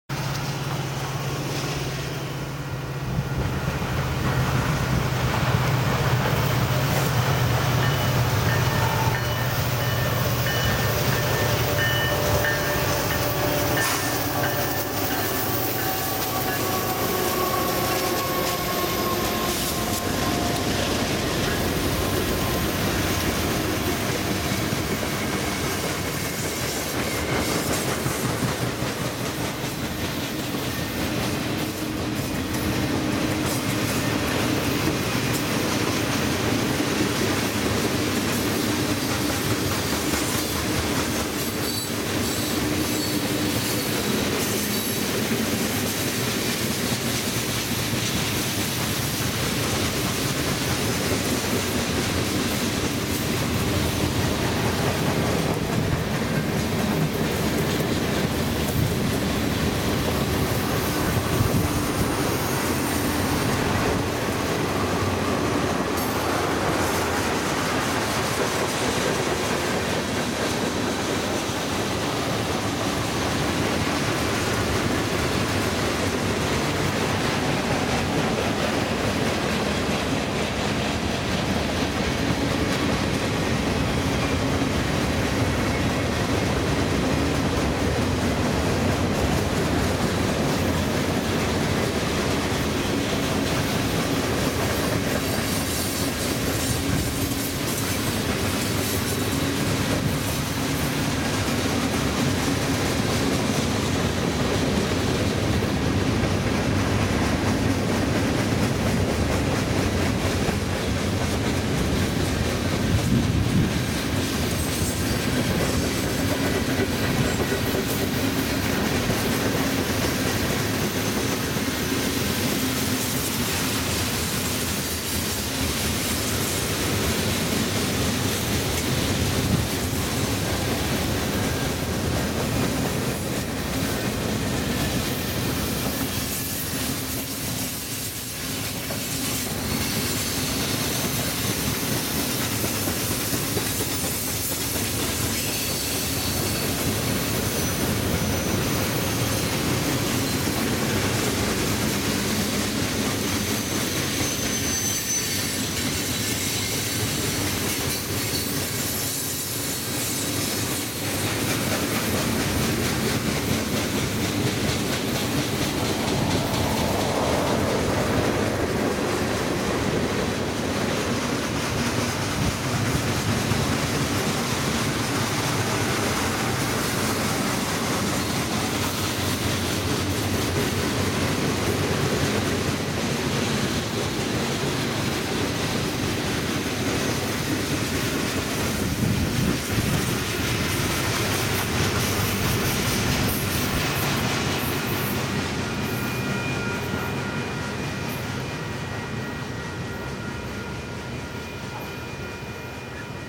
Union Pacific 8571 Leads a Mixed Freight at Rochelle,IL
UP 8571 a SD70ACe Leads a Mixed freight at Rochelle,IL on bitterly cold day.  Other power is UP7329, 4329 and CSX 5384.